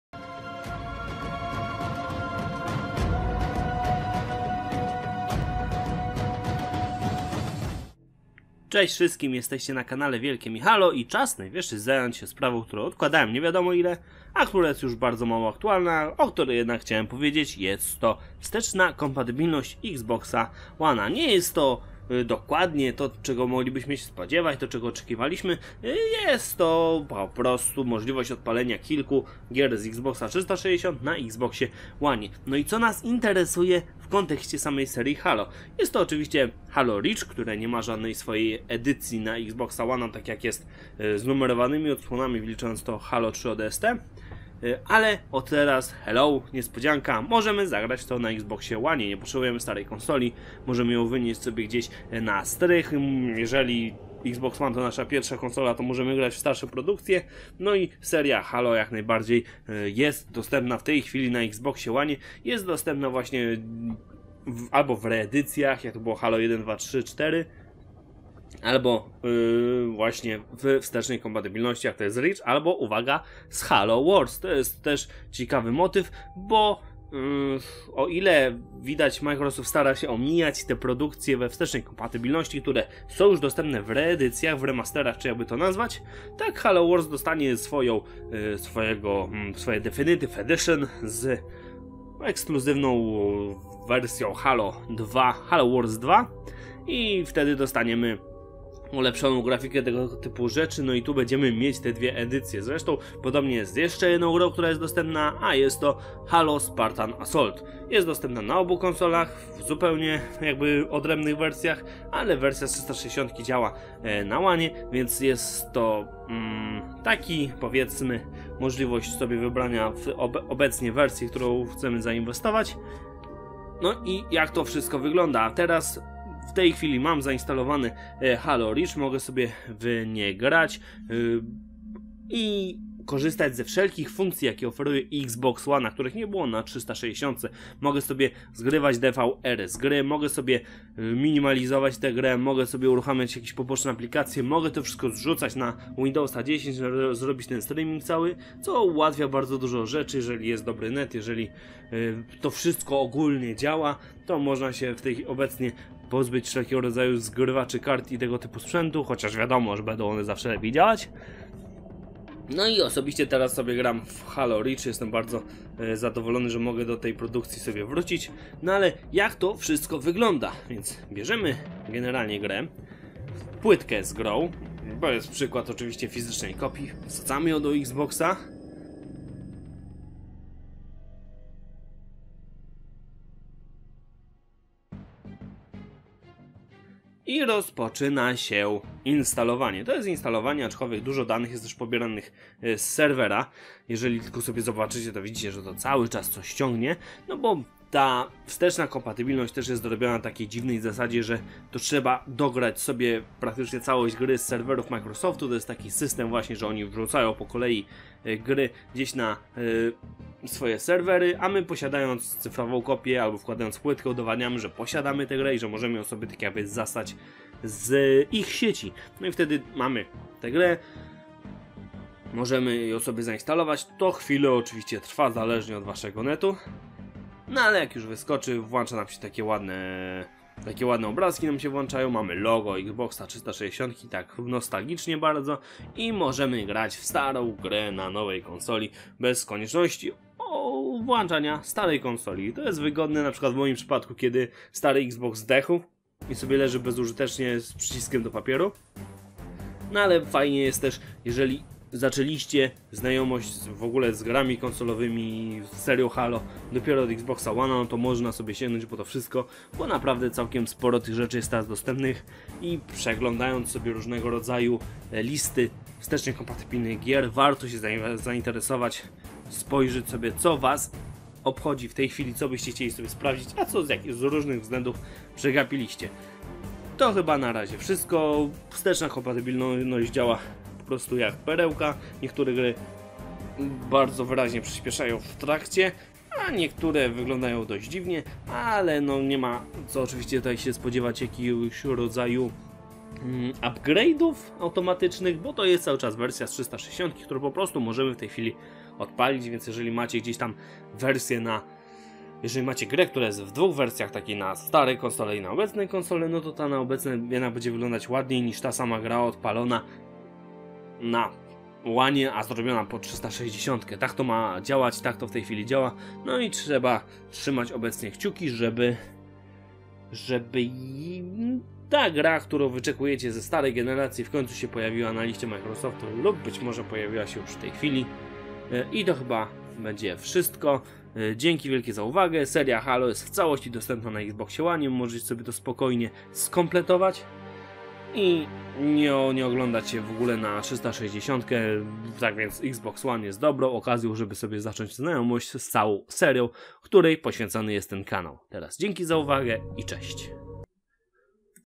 Seria Halo wstecznie kompatybilna na Xboxie One // Halog PL
Jak wygląda sprawa z graniem w starsze tytuły z serii na obecnym sprzęcie.